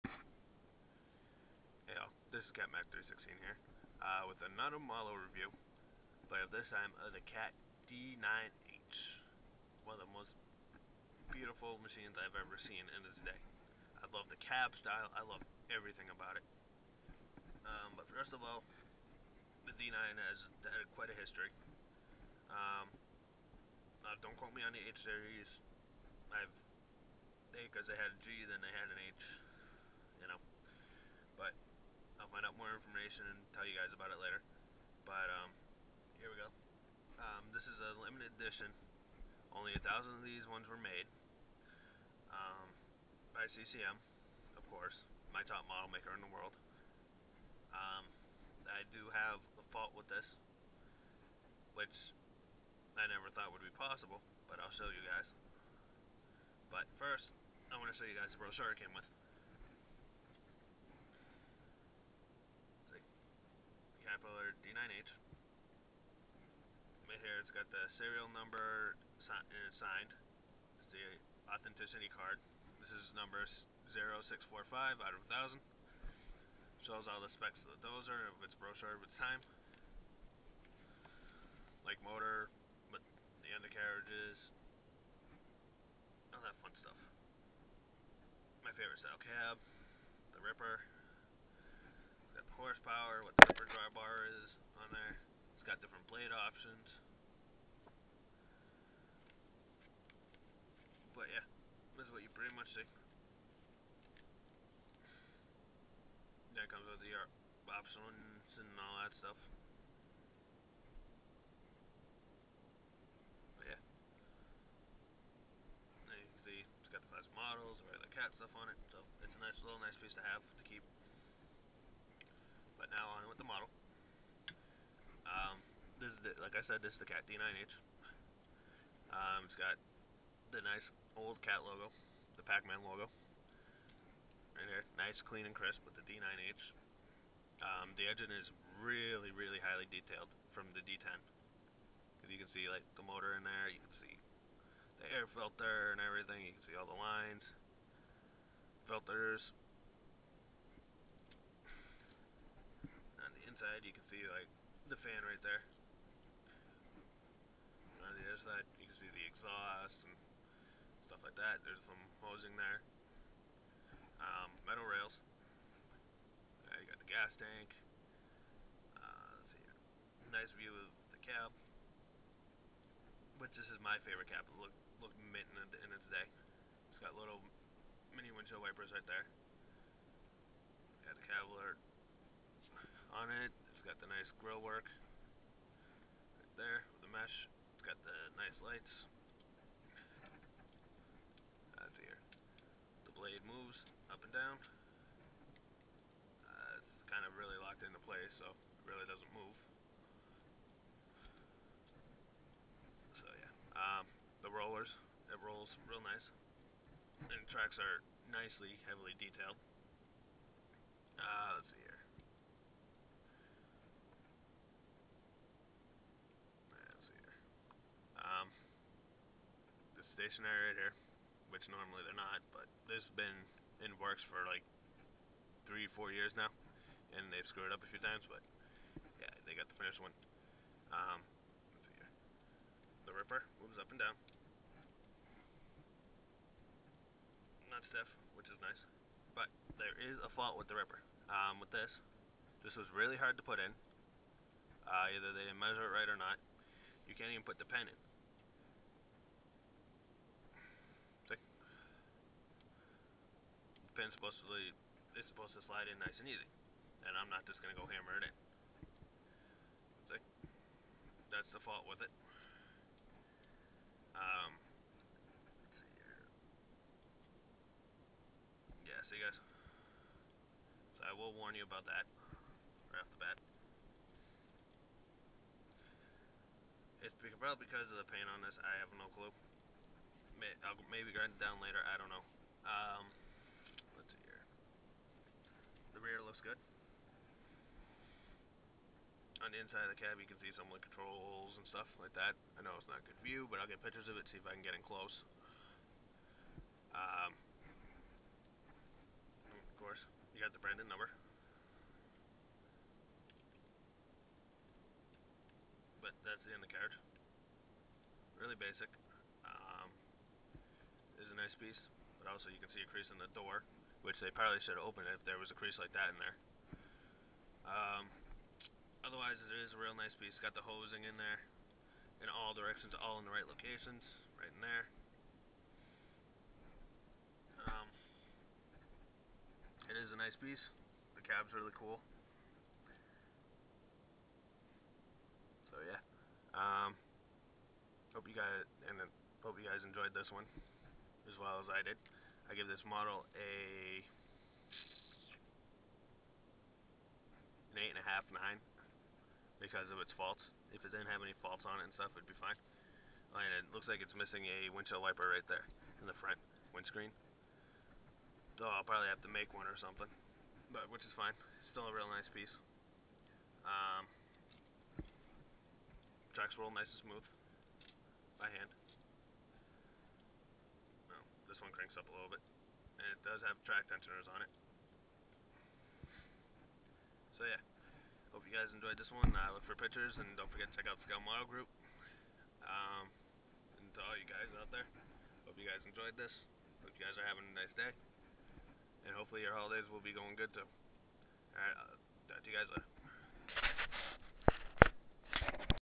CCM D9H Model review
here one my newest models its limited edtion 1000made in the version 6500n the dd9hh and 650 of the other version